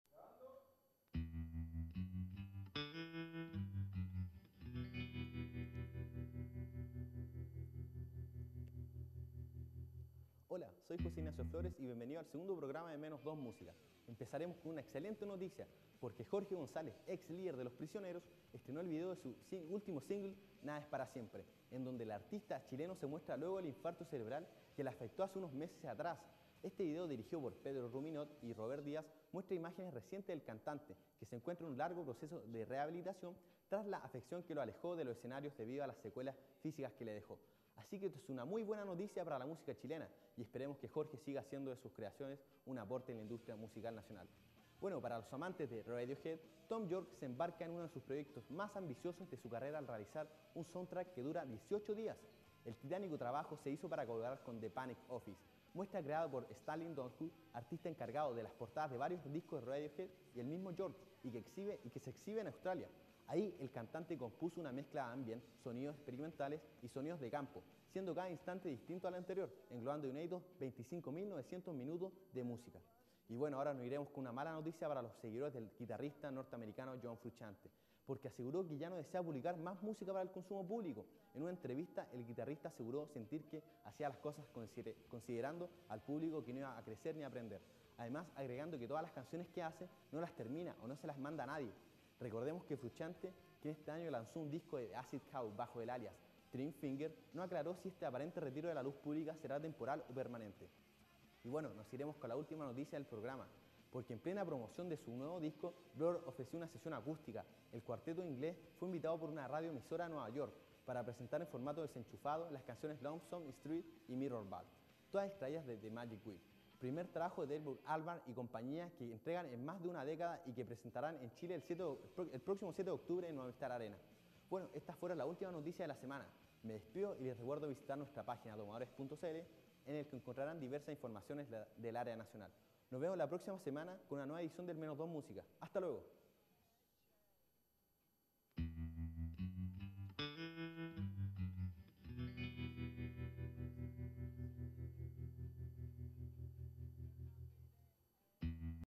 Menos 2 Música | Jorge González reaparece con nuevo vídeo musical
En otro ámbito, Thom Yorke de Radiohead graba soundtrack que dura dieciocho días, John Frusciante asegura que no desea hacer más música para el público y Blur promociona su nuevo disco en emisora de radio.